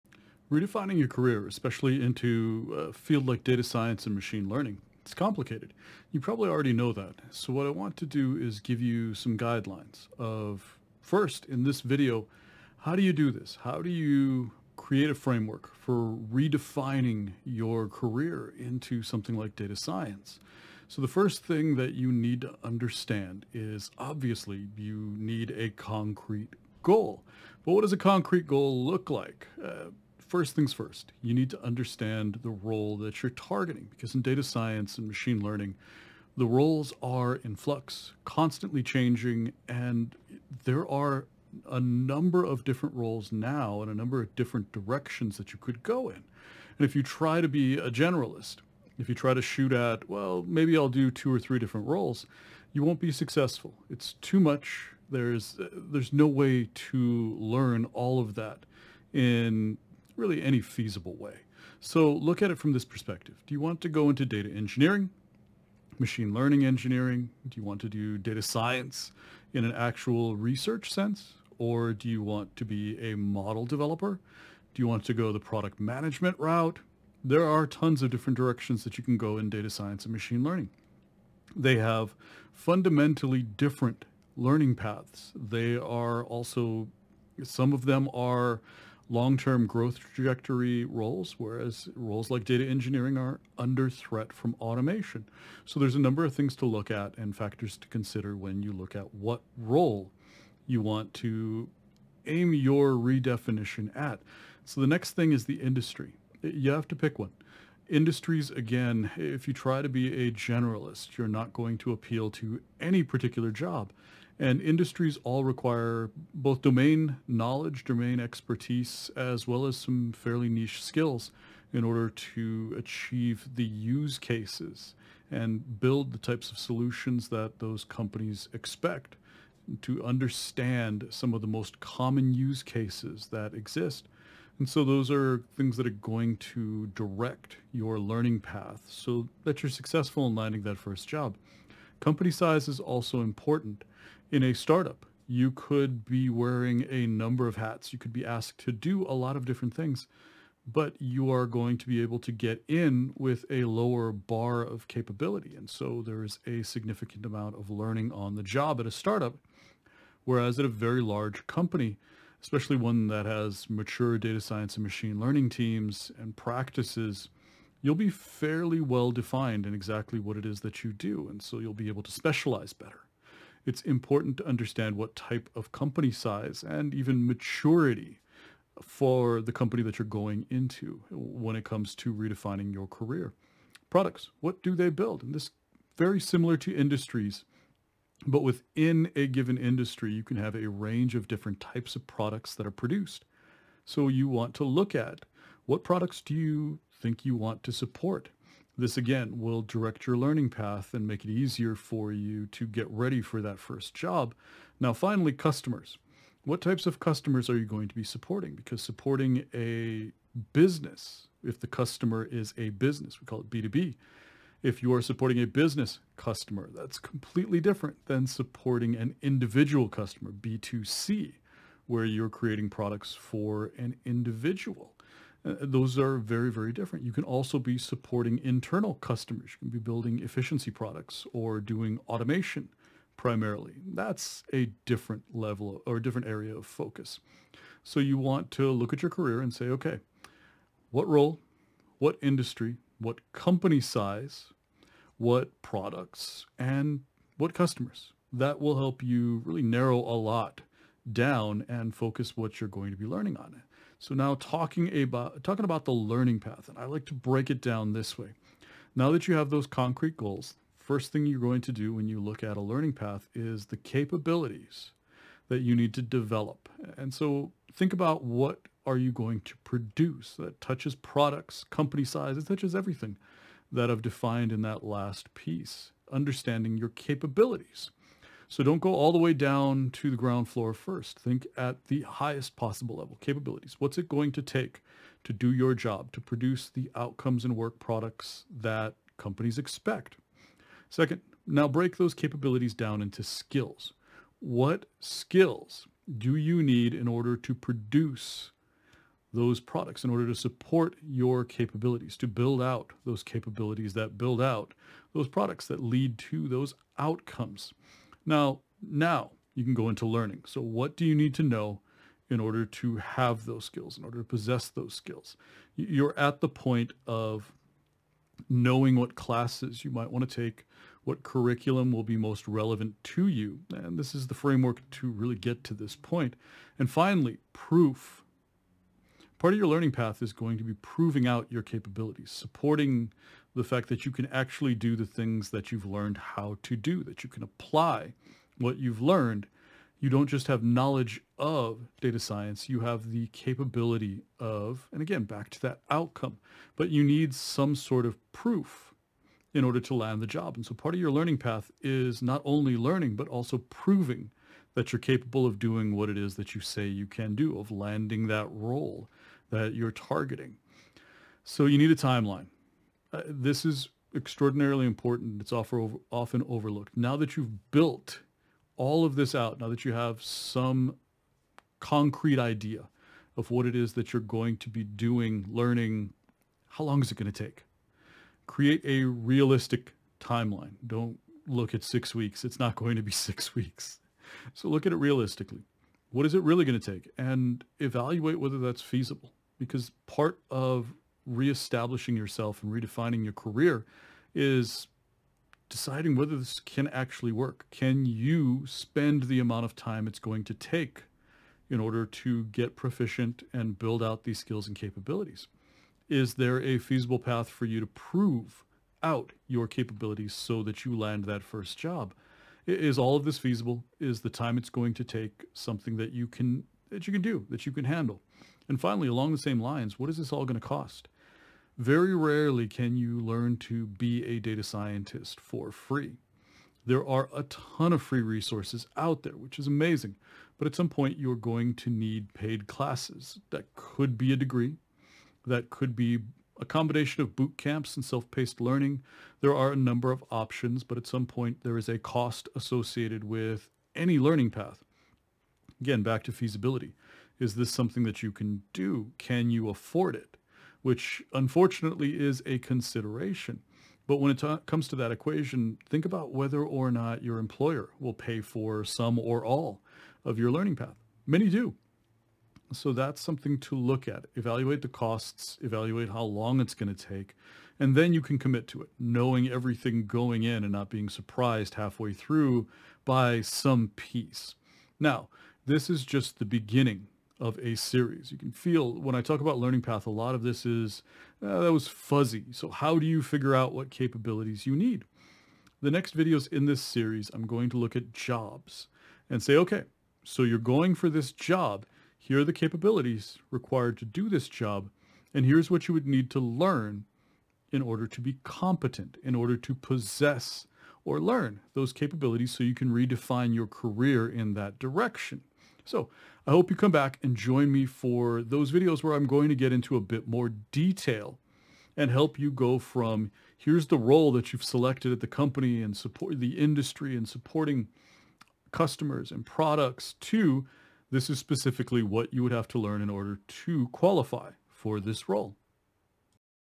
A Framework For Redefining Your Career Towards Data Science
Redefining your career towards Data Science has no easy roadmap. I have created this series to help change that. In this first video, I outline a framework for planning out your learning path. This starts with well defined goals which then help you solidify your learning path.

In subsequent videos, I am going to work backwards from a job description to a learning path.